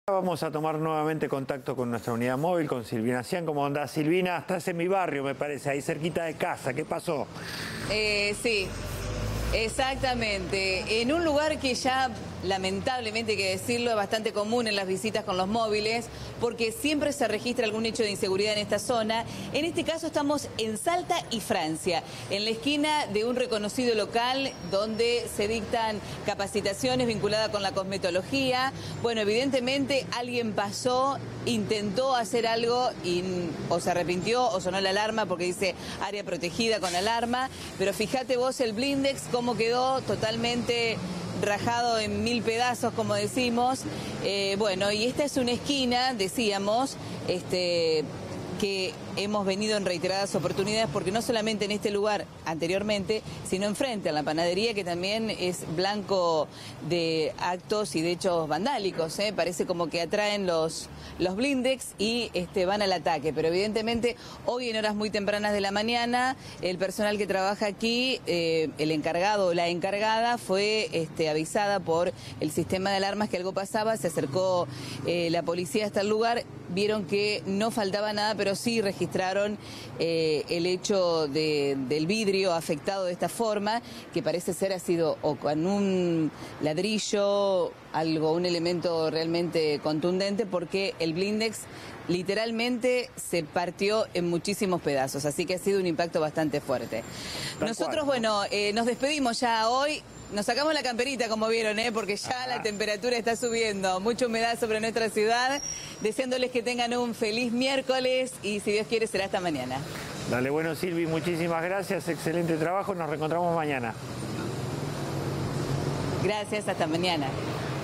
Vandalizaron un negocio en Salta y Francia
Esta mañana un blindex de un local comercial en el centro de la ciudad amaneció vandalizado. Los comerciantes están cansados de la inseguridad.